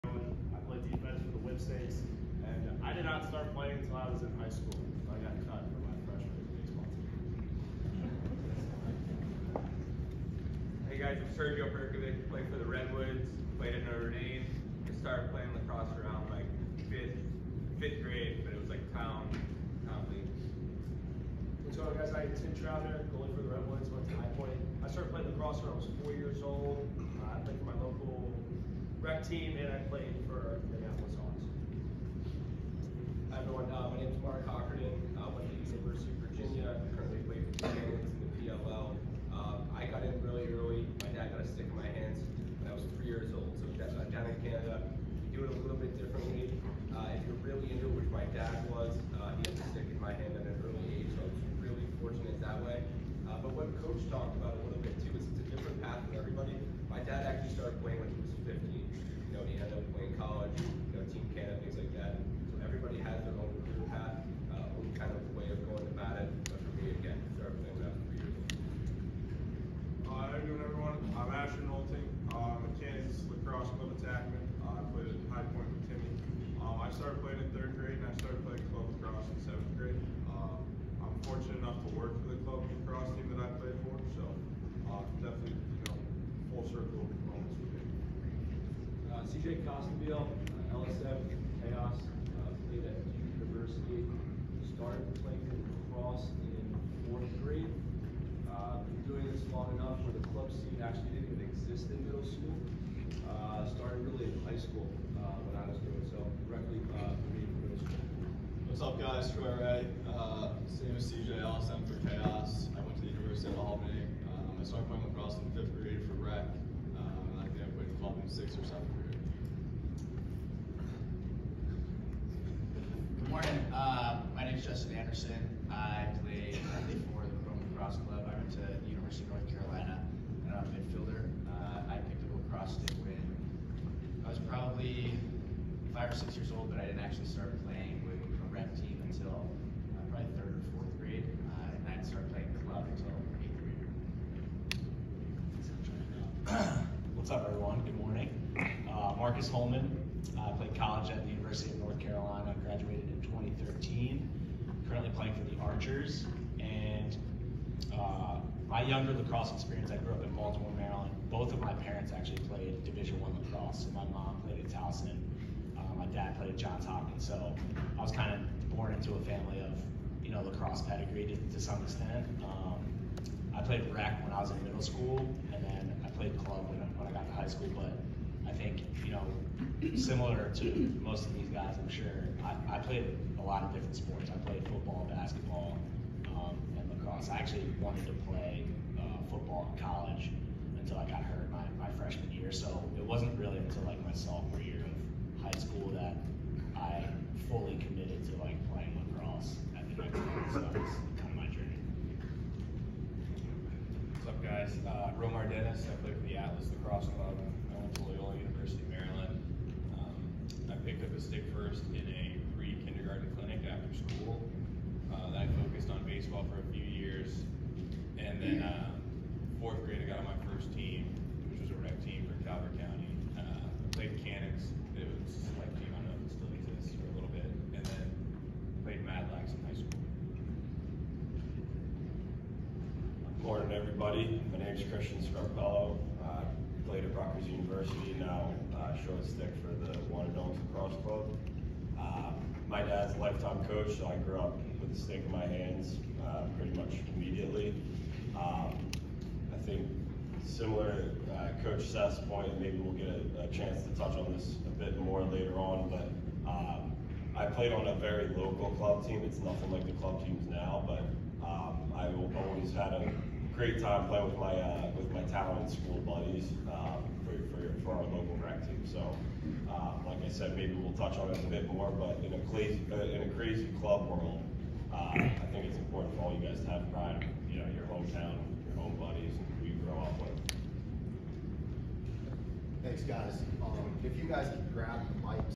PLL Jr Championship
PLL Jr Championship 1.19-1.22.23 PLL players talk

Bryce Young
Christian Scarpello
Romar Dennis
Tim Troutner
Chris Cooper
Asher Nolting
Troy Reh
CJ Costabile
Mark Cockerton
Justin Anderson
Sergio Perkovic
Andrew Kew
Marcus Holman